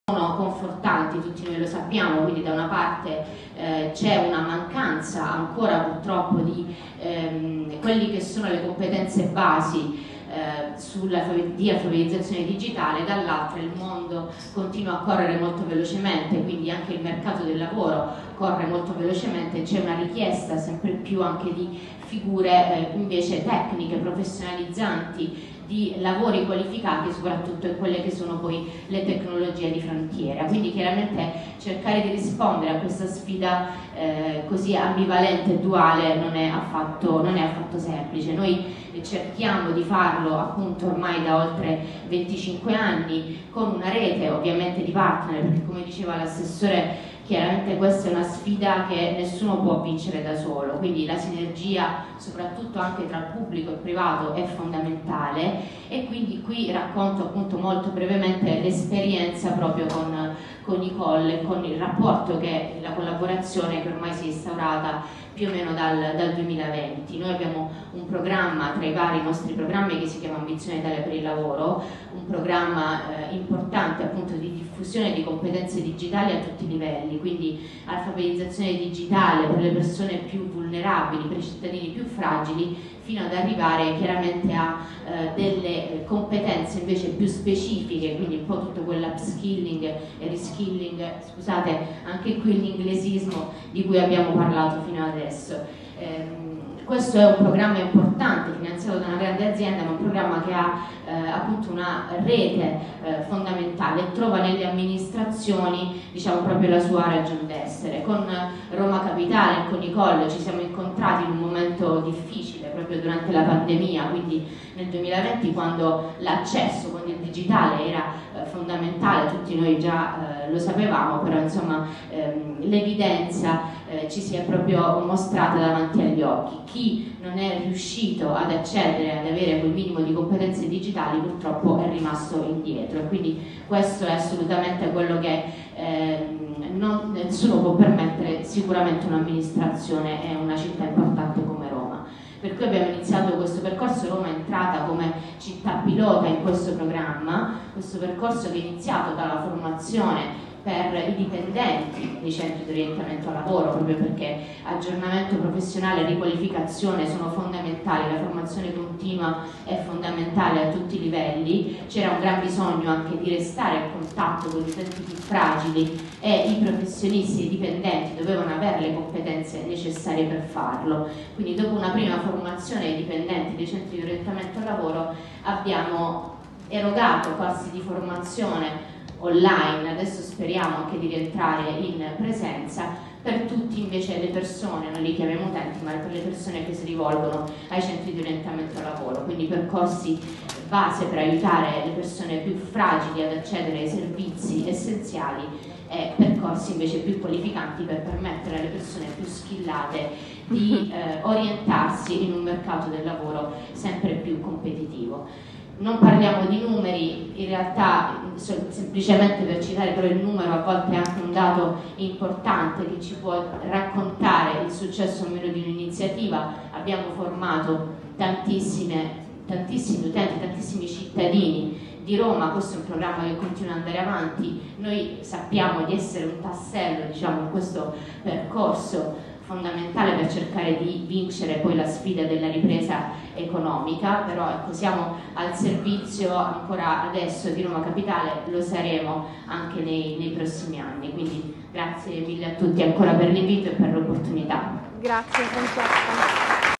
I servizi territoriali per il lavoro
La project manager Francesca Del Duca racconta la sfida del programma Ambizione Italia, realizzato con Microsoft, all'evento "Verso l'integrazione dei servizi territoriali per il lavoro",  dedicato alla Rete dei C.O.L di Roma Capitale a vent'anni dalla nascita (14 dicembre 2022).